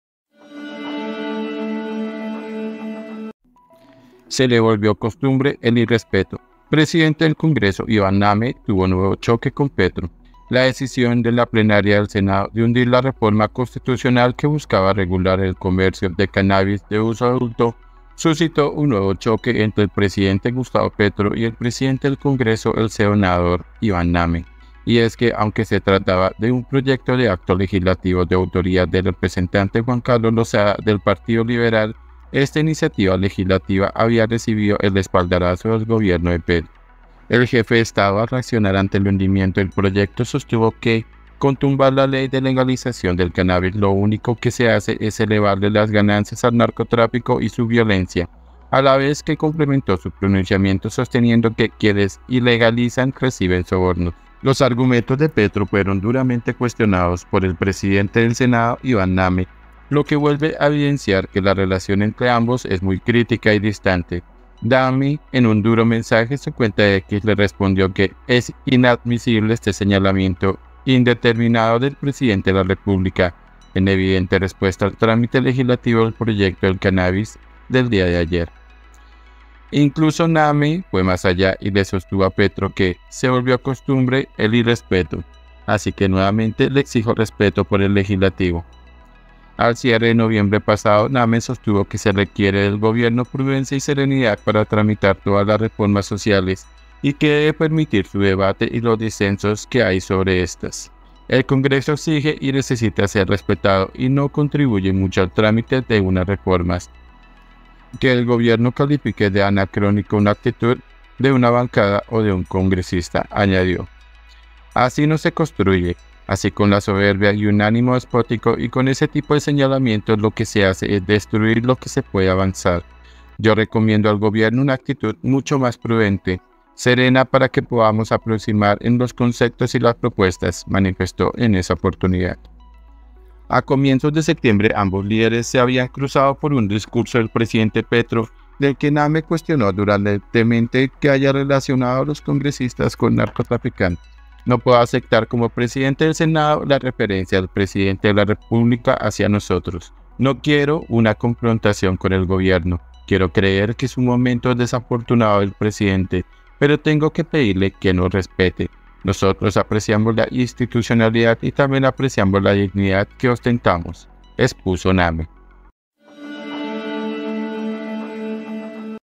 🔴 PRESIDENTE DEL CONGRESO, IVÁN NAME, TUVO NUEVO CHOQUE CON EL PRESIDENTE PETRO
La decisión de la plenaria del Senado de hundir la reforma constitucional que buscaba regular el comercio de cannabis de uso adulto, suscitó un nuevo choque entre el presidente Gustavo Petro y el presidente del Congreso, el senador Iván Name.
EL COLOMBIANO